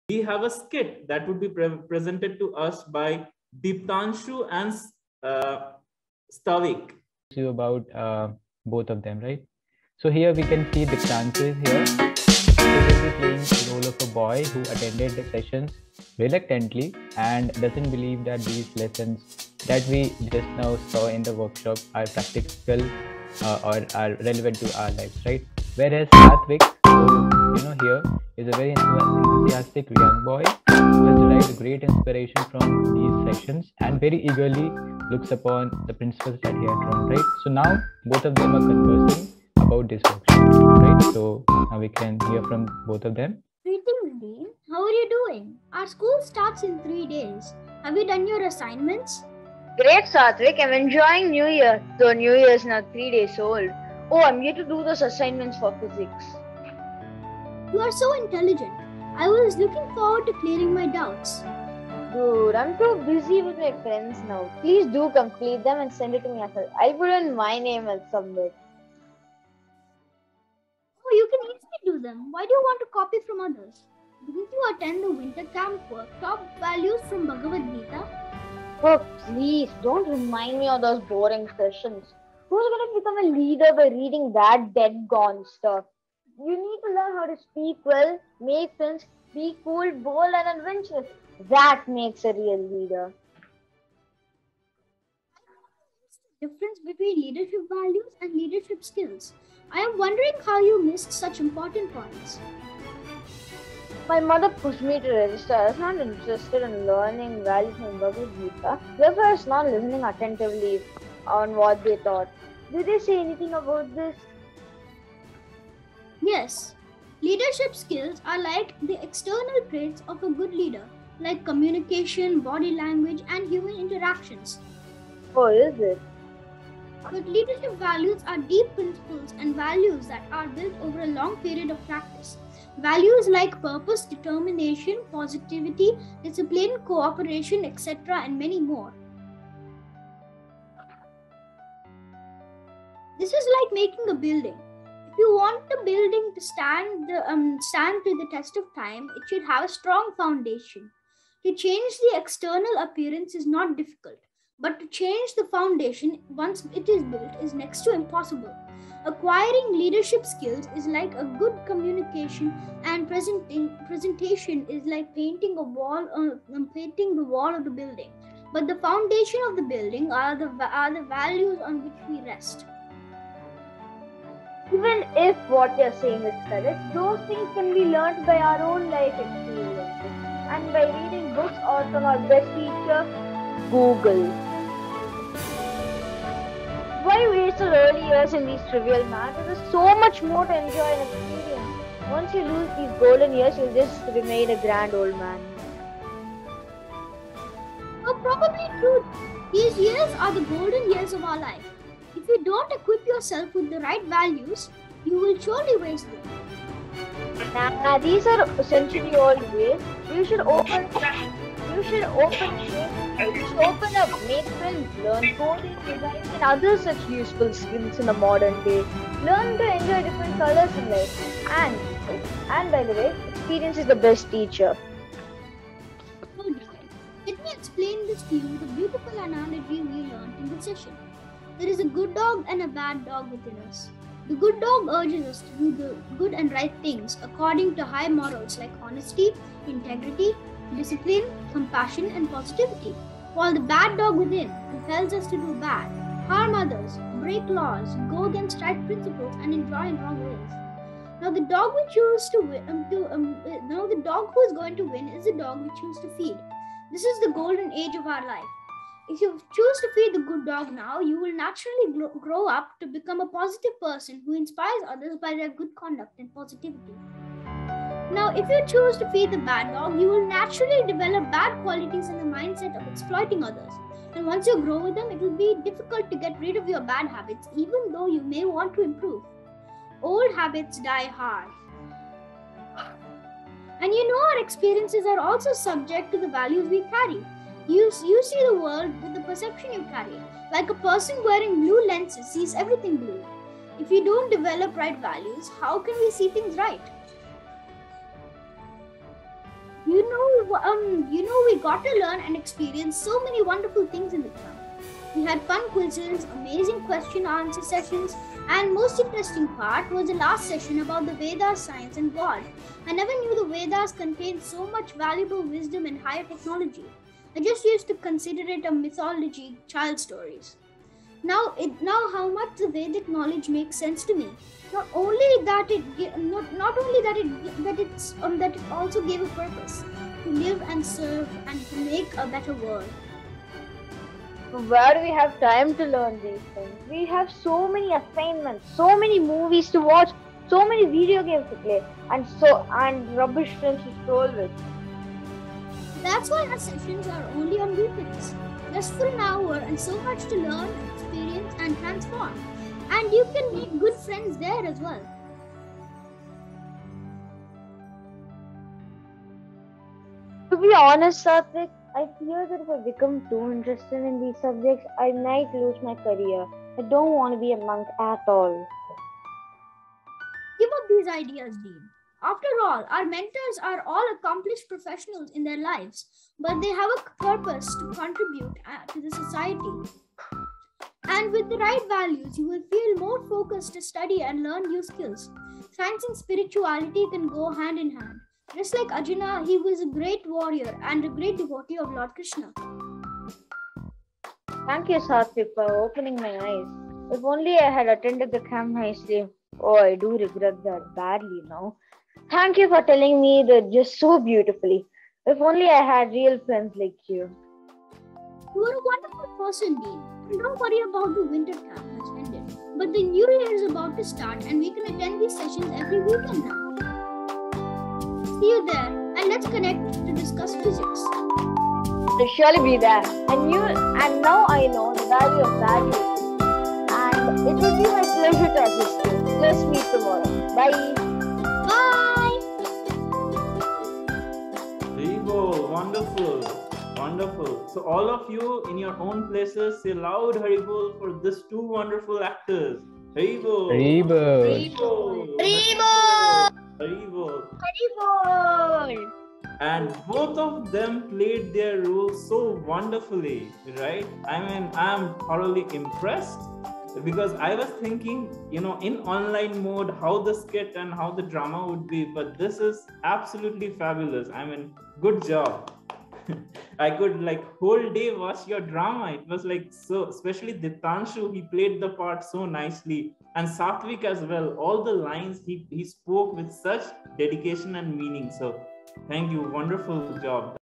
Value of Cultivating Leadership Values - Winter Camp Drama
Wonderful skit presented by our Value Leaders Diptanshu from Bangalore and Satvik from UAE .